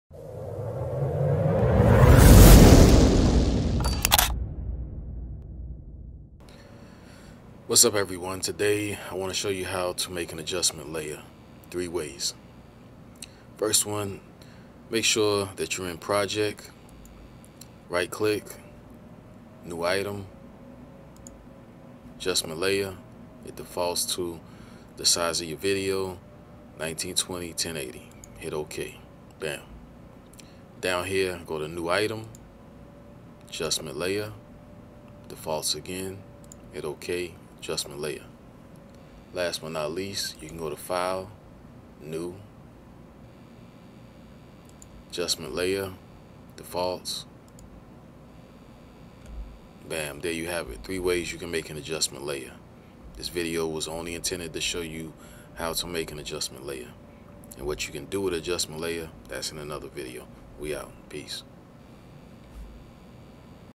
How to create an adjustment layer in Adobe Premiere Pro
A video about creating an adjustment layer in Adobe Premiere Pro